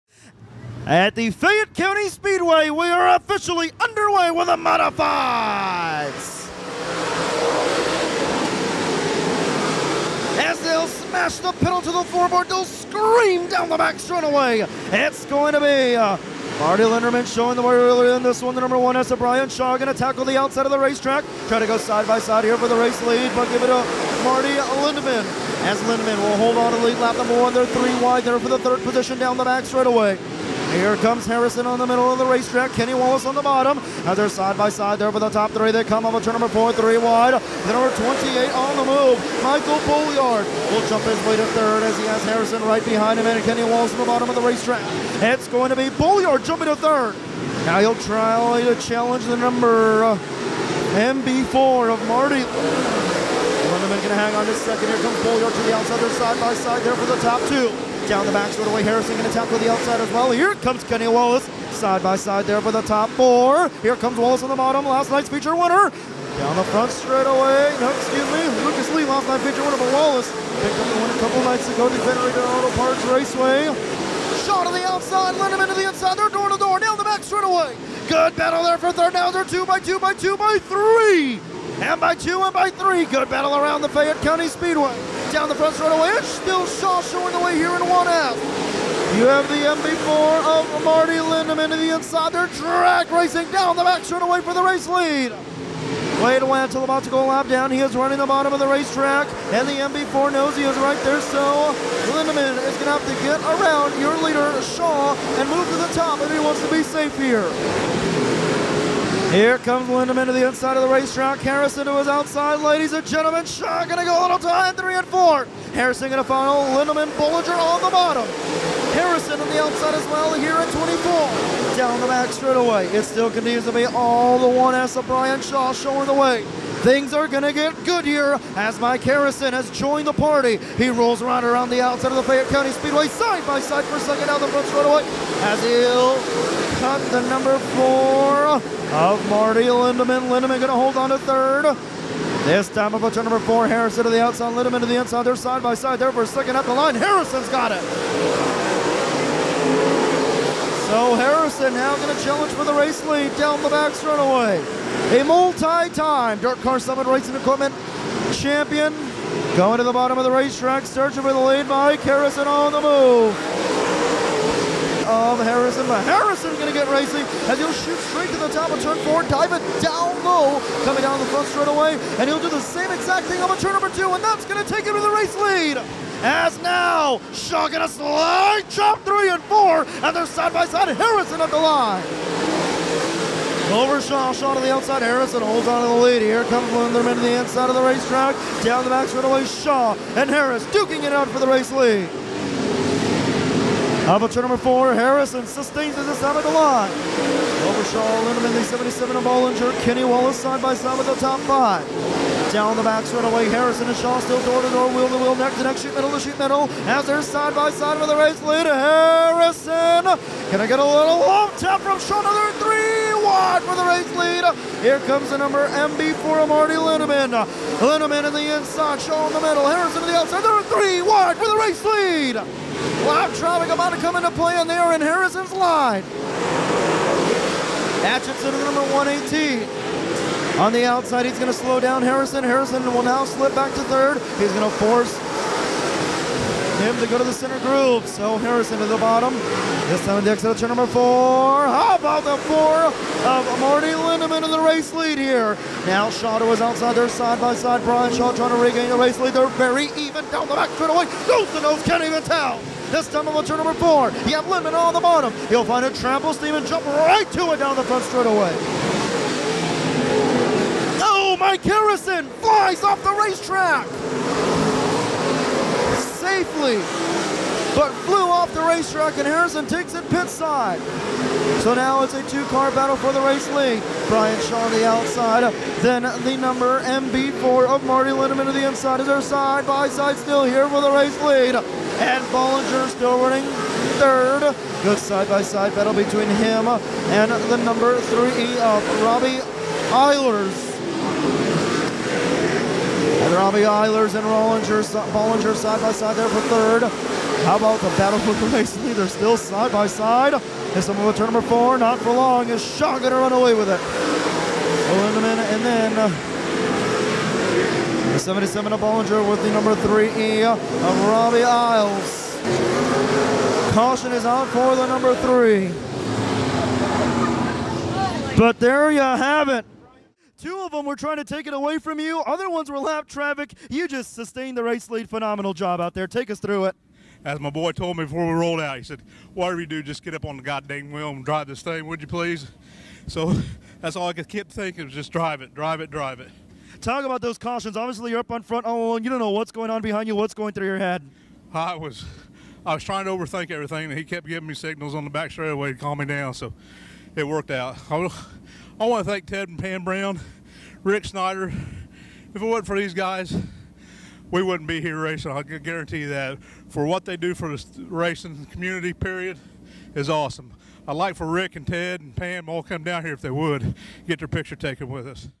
DIRTcar Summer Nationals Modifieds Fayette County Speedway July 4th, 2018 | HIGHLIGHTS
DIRTcar Summer Nationals Modifieds from Fayette County Speedway on July 4th, 2018.